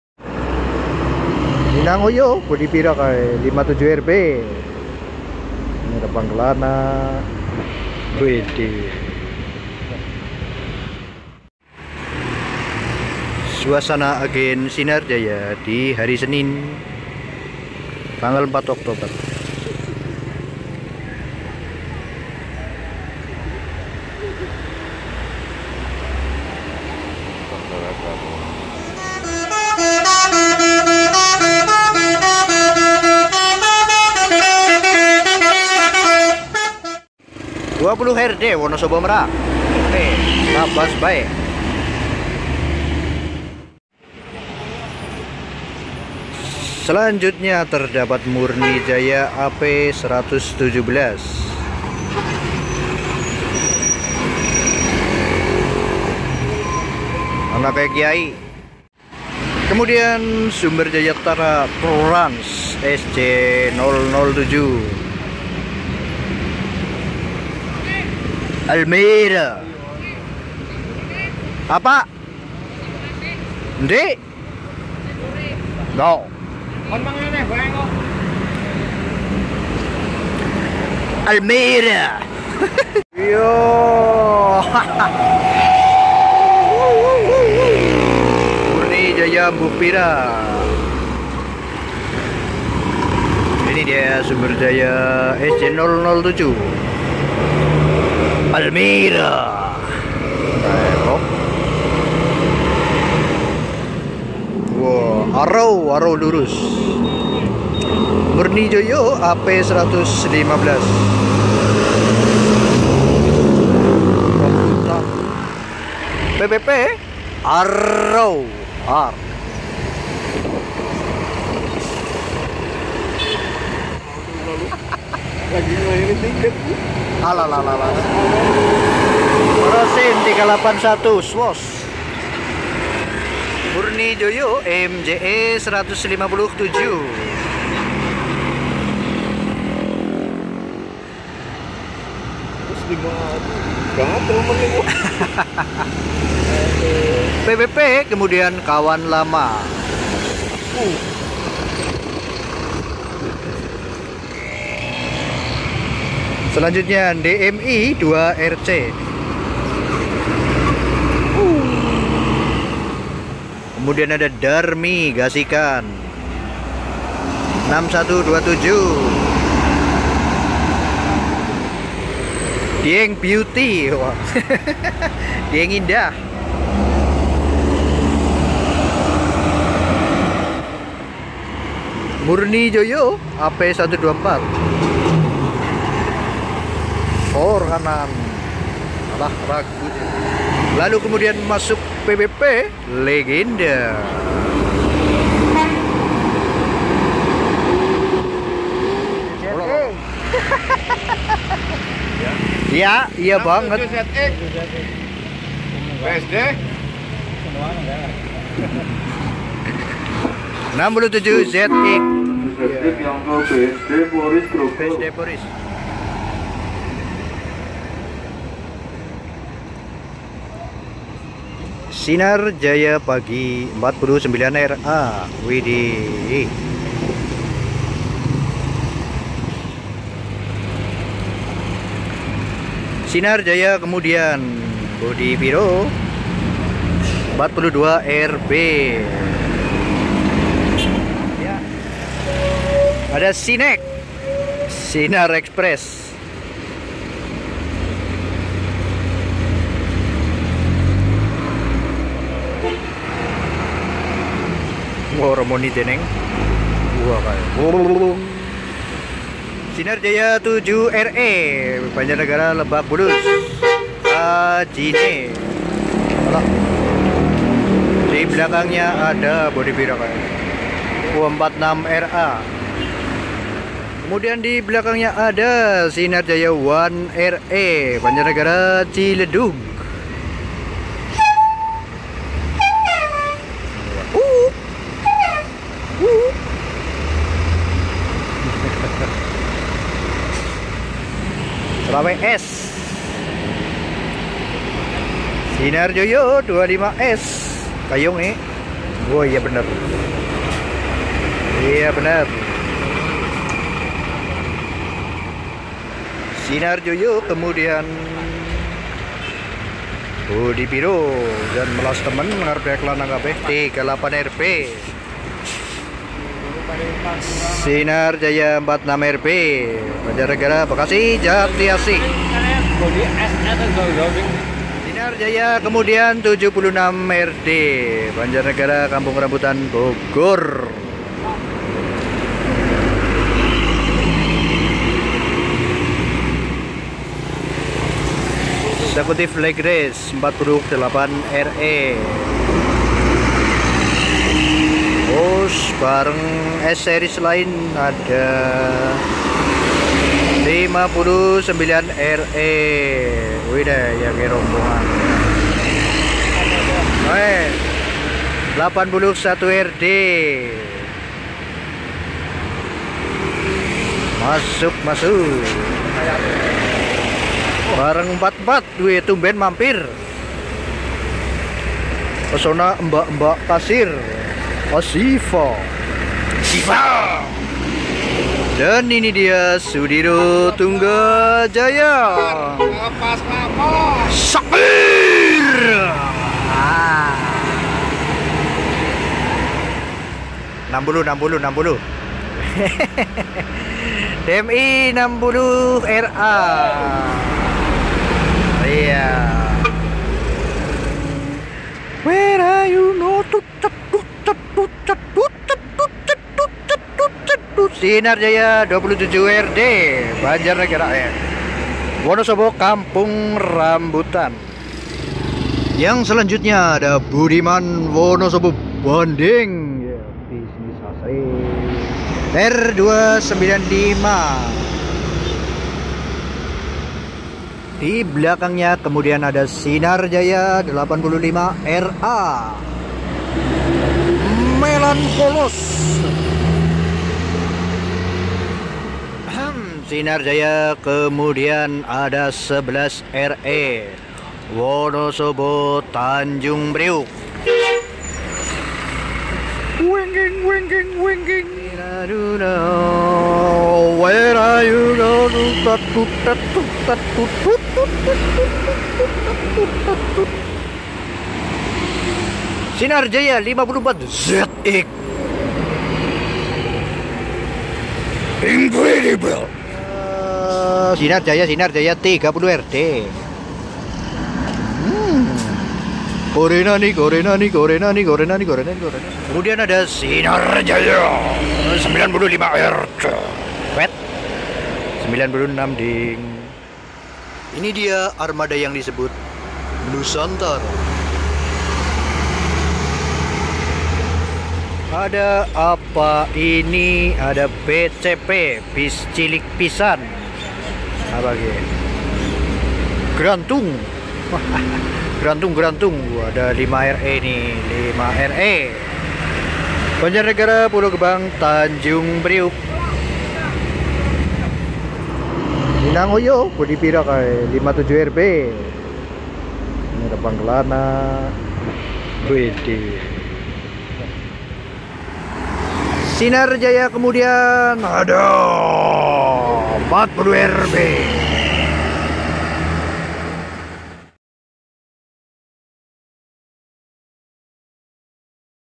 Beda Suasana Akhir Pekan dan Hari Biasa. SJT 007 ALMEERA, STJ SHAKEER Lebih Bersemangat ‼️
Assalamu'alaikum minasan

Ok selamat datang di kukuh 26 channel

Pantauan kali ini seperti biasa di klampok area, untuk melihat perbedaan suasana agen klampok, Sinar Jaya terutama...hehehe, antara hari biasa dan akhir pekan atau awal pekan mungkin ya, kalau hari minggu. 😁